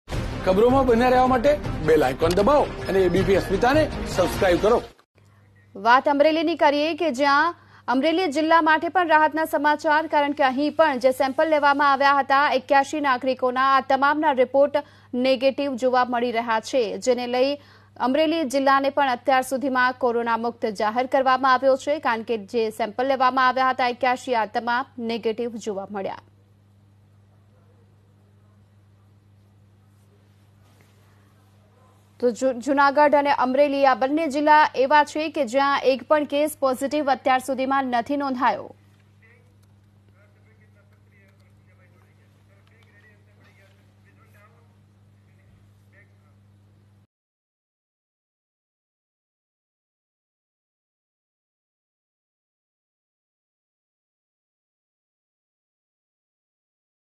All 81 Samples Of Covid-19 Are Negative In Amreli | ABP Asmita
All  81 samples of Covid-19 are negative in Amreli